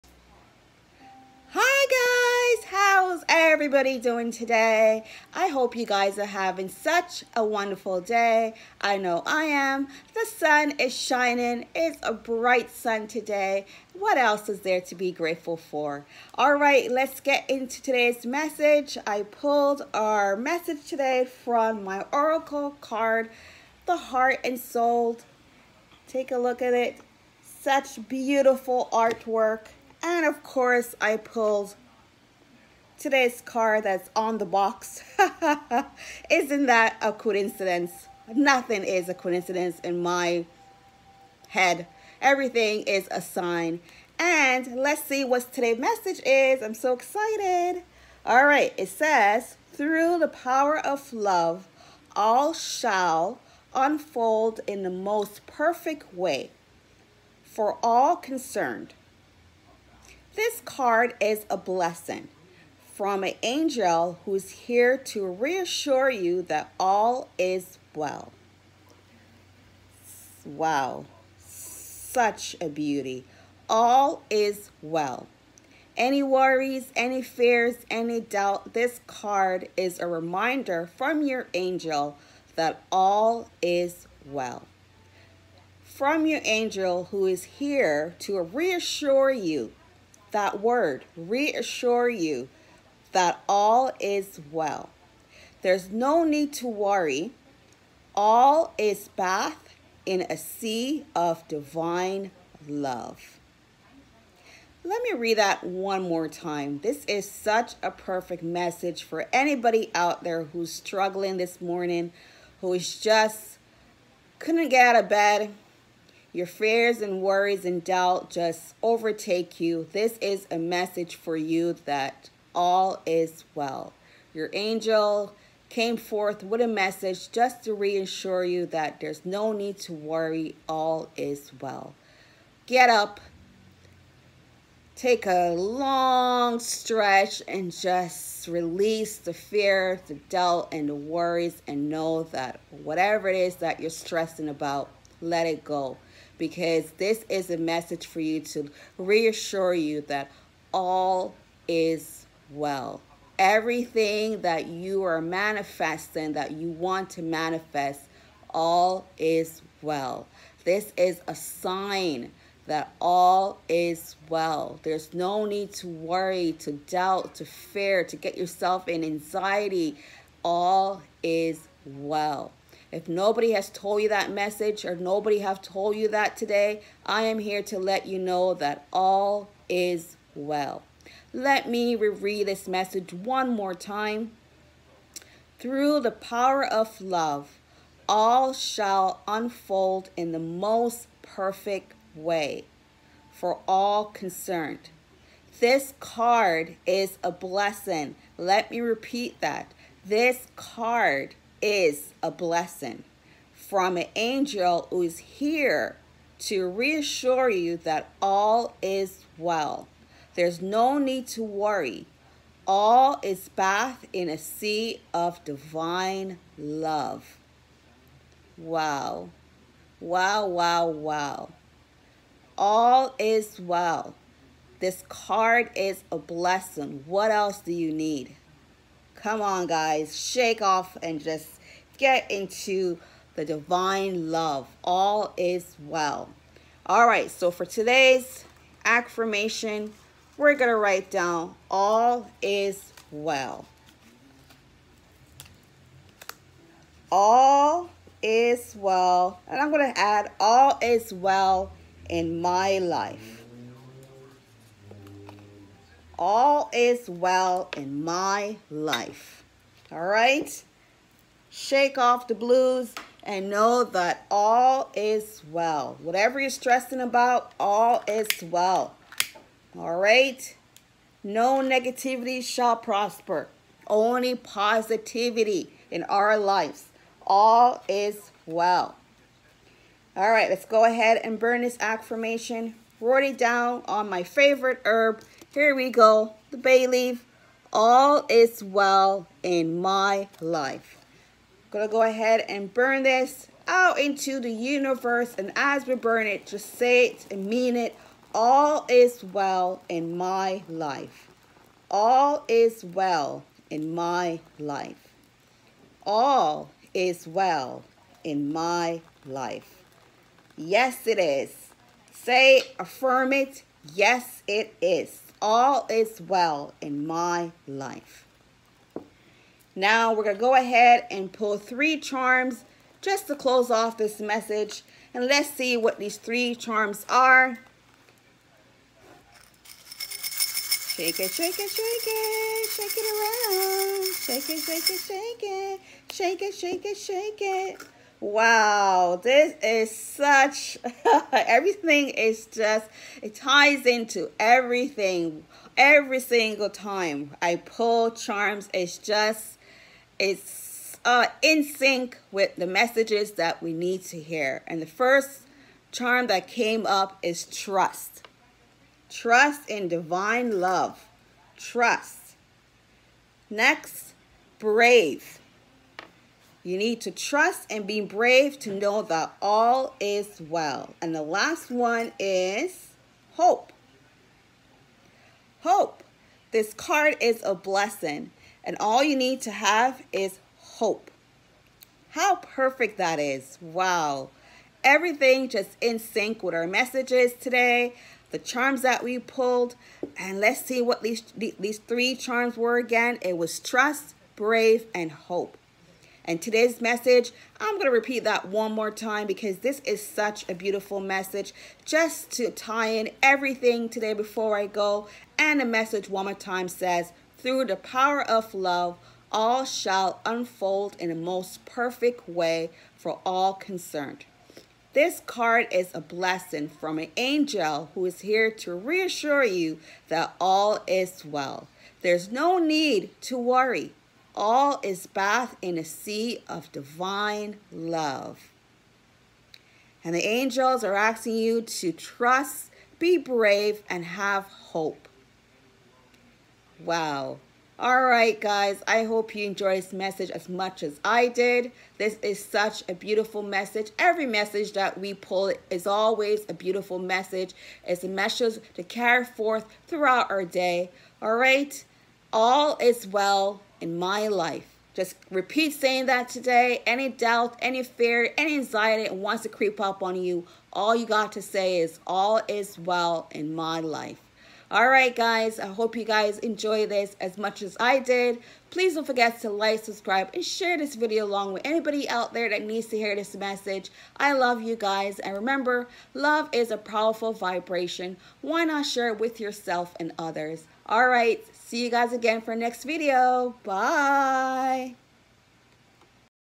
THIS MESSAGE IS A BLESSING✨ | BURNING BAY| CHARMS
Message of the day:
There is no need to worry. All is bathed in a sea of divine love.

Affirmation of the day:
All is well in my life

Happy Blessings!